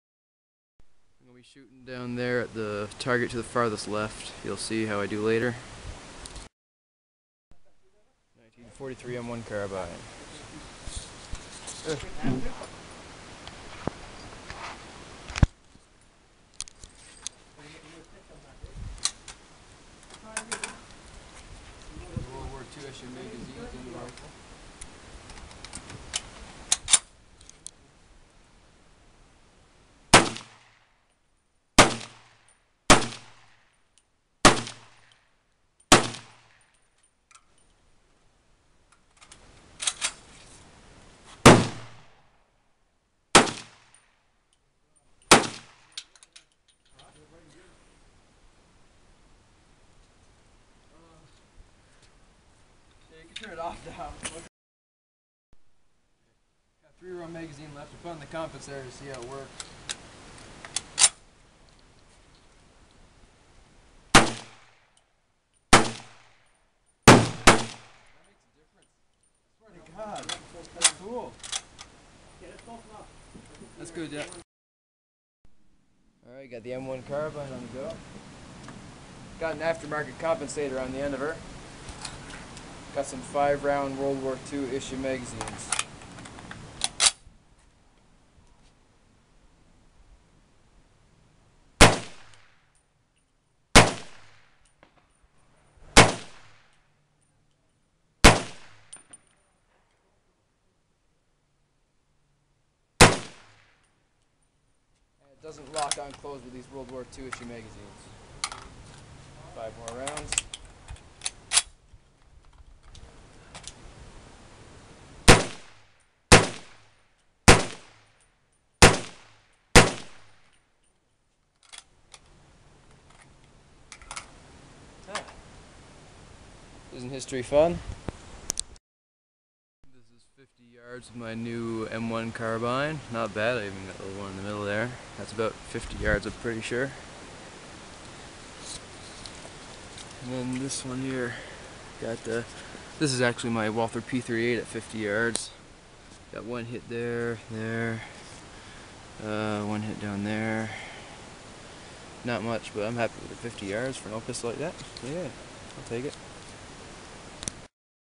Range Time Sept 6 2010
First time out with the M1 Carbine, we did a lot more shooting than just this lol The front hand guard popped off because I didn't have the barrel band screwed on tight enough. I found the compensator did make a difference, even though the .30 Carbine has very little recoil to begin with.